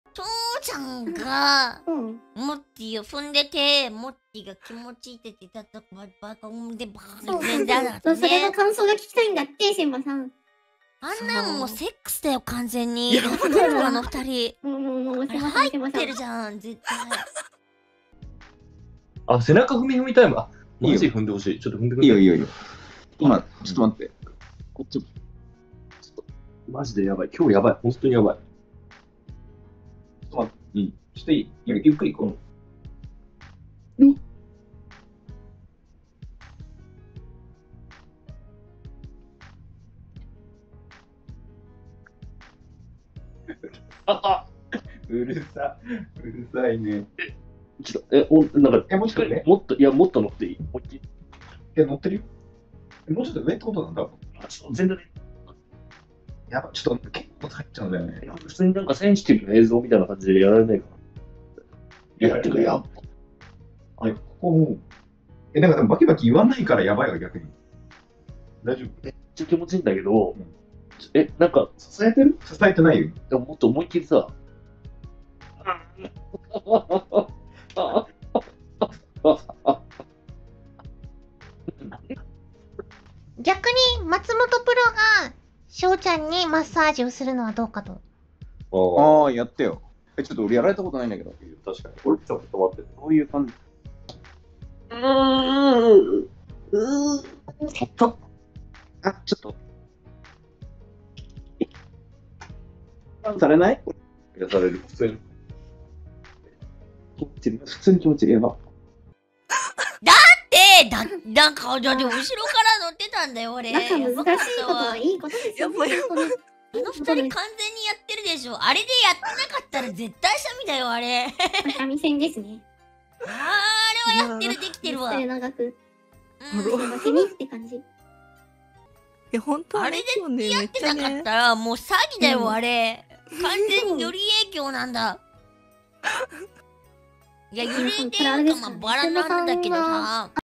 【切り抜き】白鳥翔と松本吉弘が完全にセ●クスしていると思ってる千羽 黒乃
▼元動画様
【Music】
Madeon

▼素材
七三ゆきのアトリエ